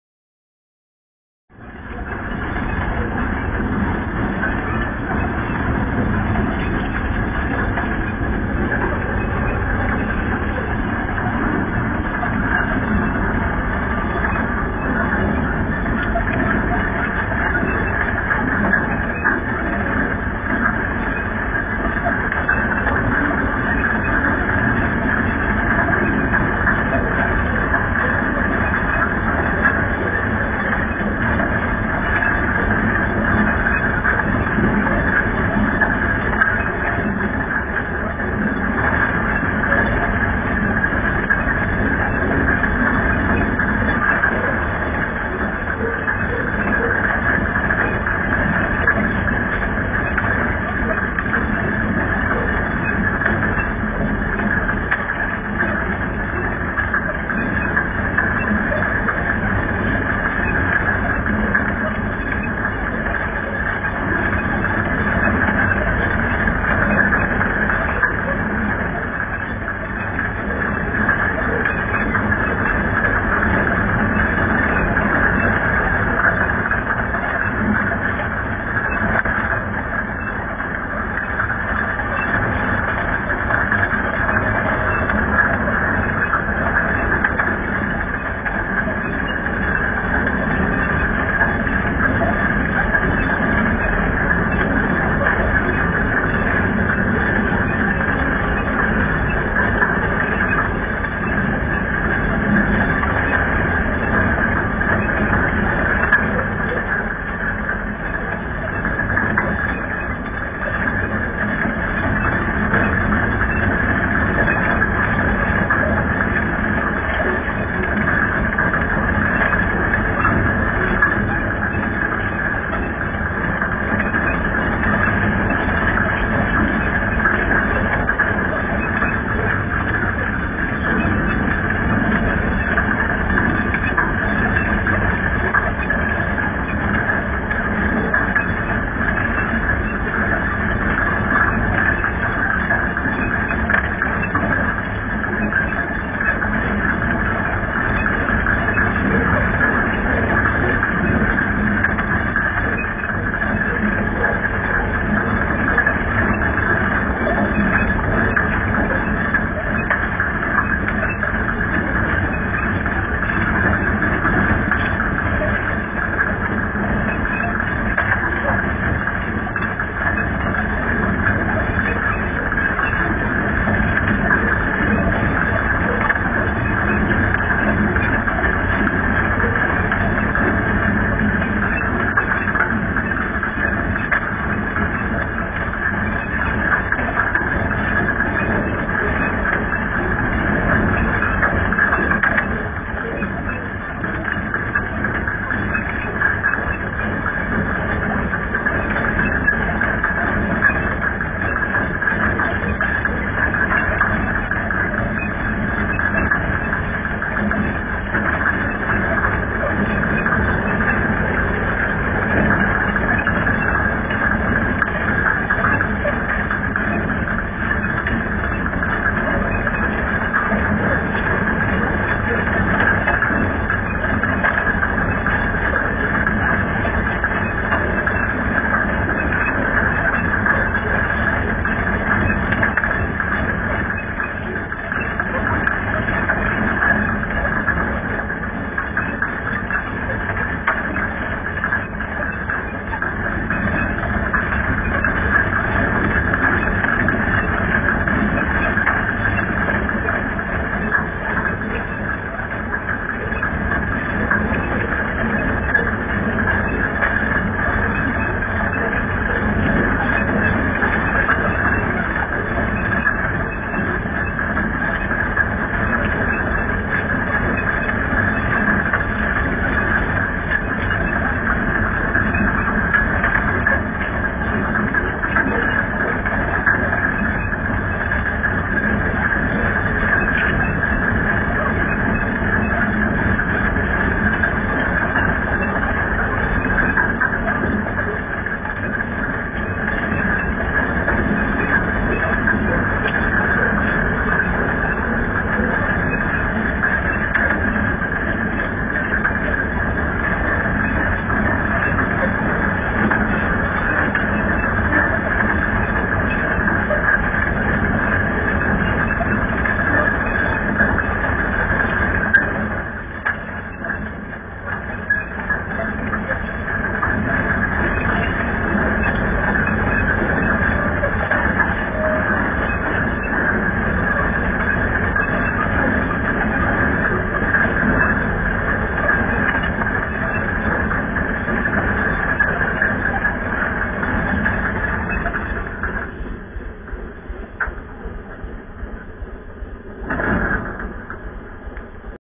Bulvės sezonas 2015
gyčio norvilo vaizdas (2015 rugsėjo 13 d. Pandėlys, Cvirkos gatvė)